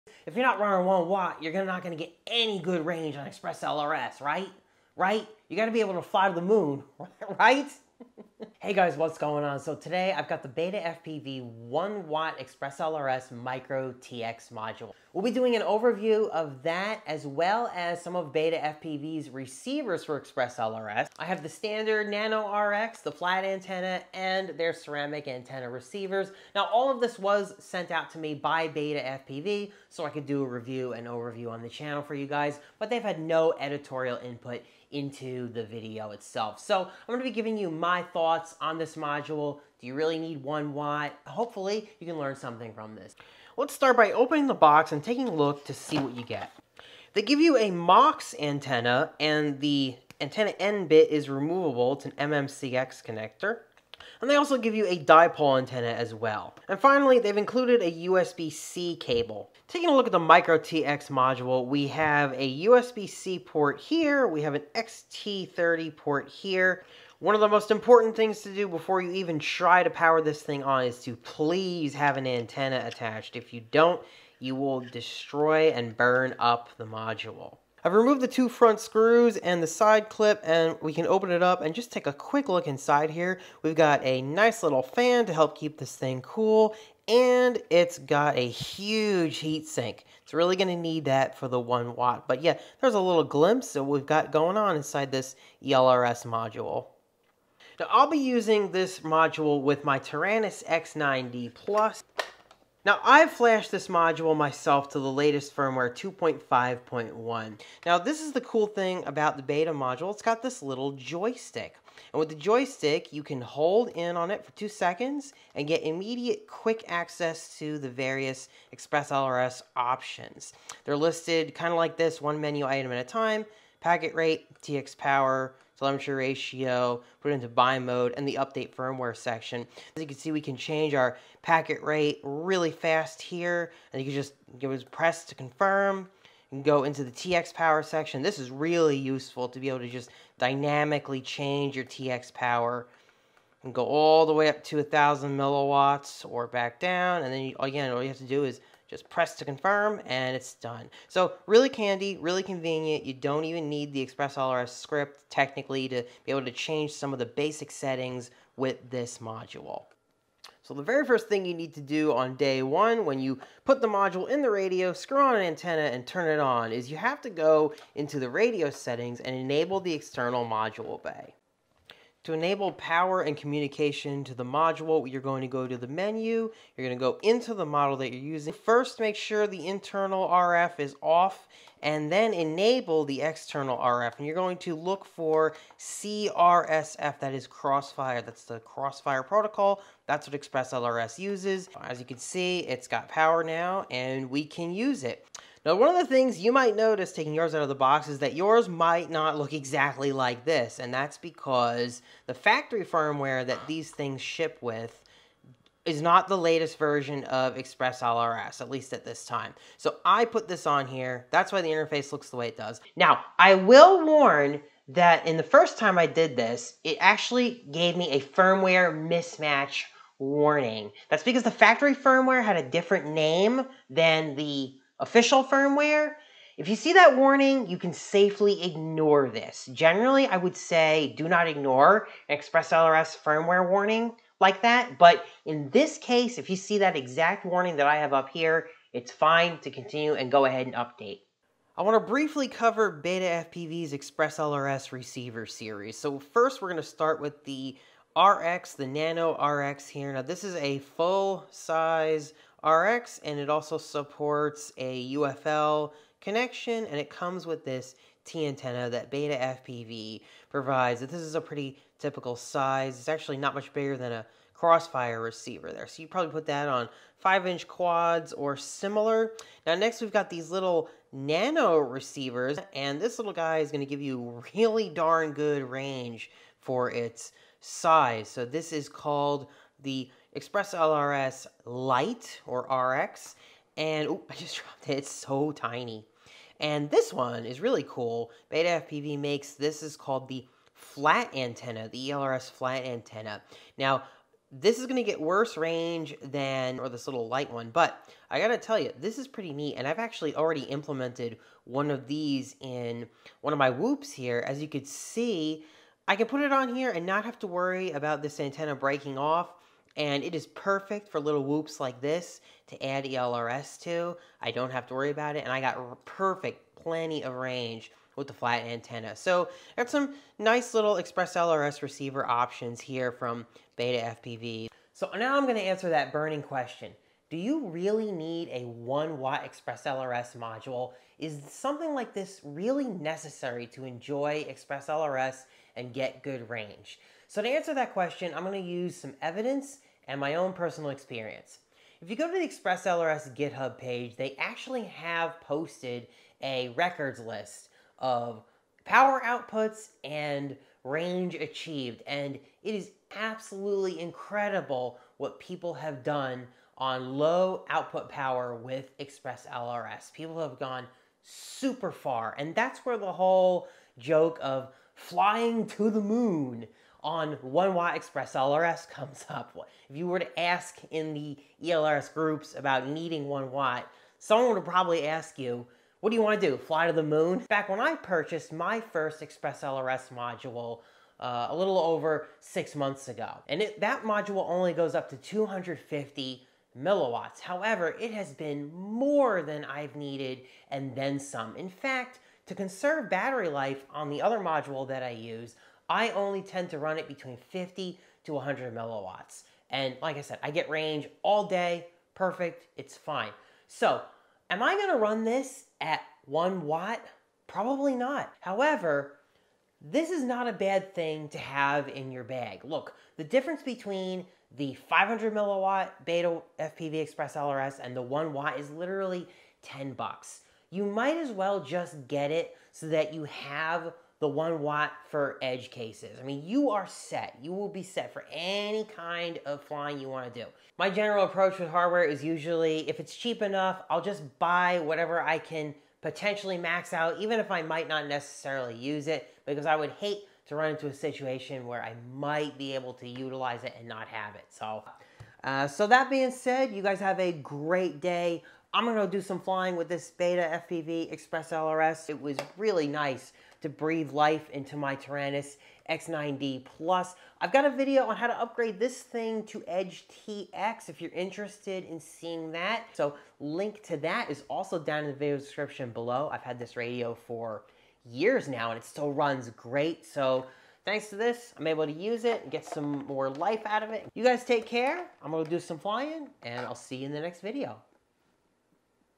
BetaFPV ELRS Micro TX 1000mw & Receivers
📶 BetaFPV ELRS Micro TX Module 1000mw

-  Intro to BetaFPV's 1W ExpressLRS... To The Moon?: 0:00
-  Antennas, Cables, and Module: 0:43
-  Do NOT power on with NO Antenna: 1:05
-  Inside the Module: Fan & Heat Sink: 1:17
- Joystick Interface Support & OLED Screen: 1:38
- General Setup in EgdeTX (Taranis XD9+): 2:40
- Firmware Upgrade Tip & Target Warning: 3:16
- BetaFPV's ELRS Receivers Overview: 4:05
- So Do You Really NEED 1W? Probably not, but... : 5:25
- 1W vs 500mw Cost Is Insignificant...: 6:48
- Outtro: 7:25